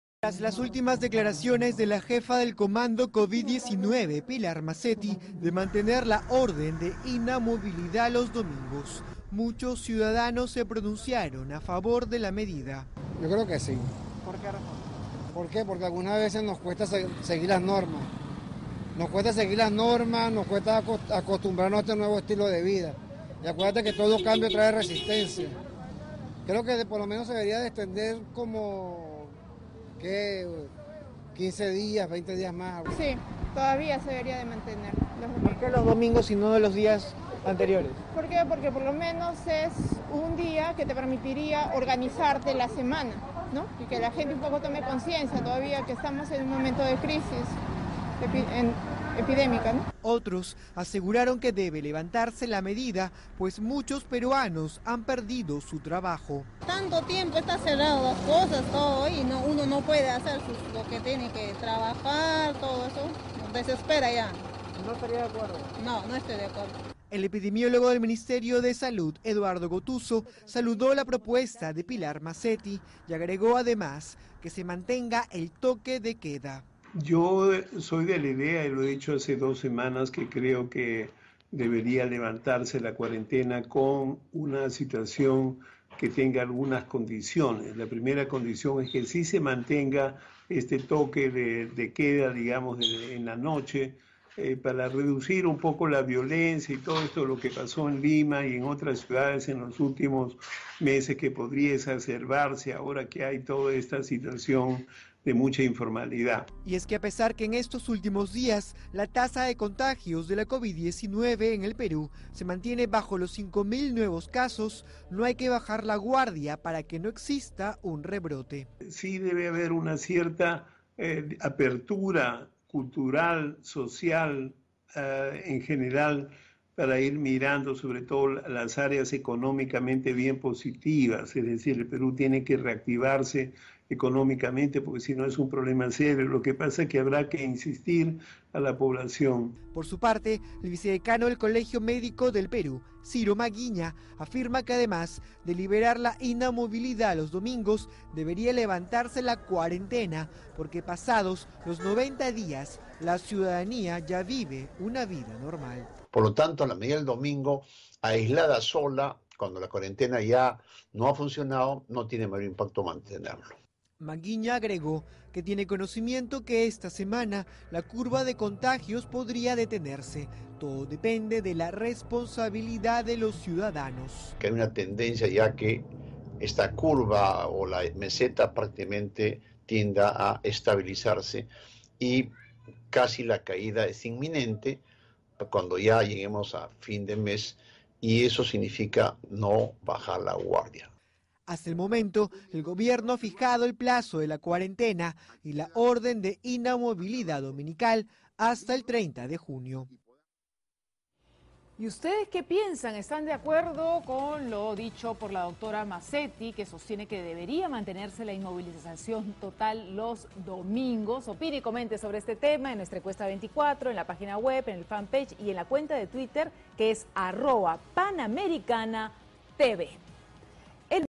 Jefa del Comando COVID-19 pide mantener inmovilización social los domingos
La jefa del Comando COVID-19, la Dra. Pilar Mazzetti solicita mantener la orden de inmovilización social obligatoria los días domingos, como medida extraordinaria para detener la propagación del coronavirus que ya cobra la vida de 7.056 personas en nuestro país.

Por su parte, el epidemiólogo del Ministerio de Salud (Minsa), el Dr. Eduardo Gotuzzo saludó la propuesta de la Dra. Pilar Mazzetti e indicó que además es importante que se mantenga el toque de queda.

El vicedecano del Colegio Médico del Perú, el Dr. Ciro Maguiña señala que, además de liberar la inamovilidad los días domingos se debería levantar la cuarentena porque pasado los 90 días de aislamiento social la ciudadanía ya vive una vida normal.

Hasta el momento el Gobierno ha fijado el plazo de la cuarentena y la orden de inamovilidad dominical hasta el 30 de junio.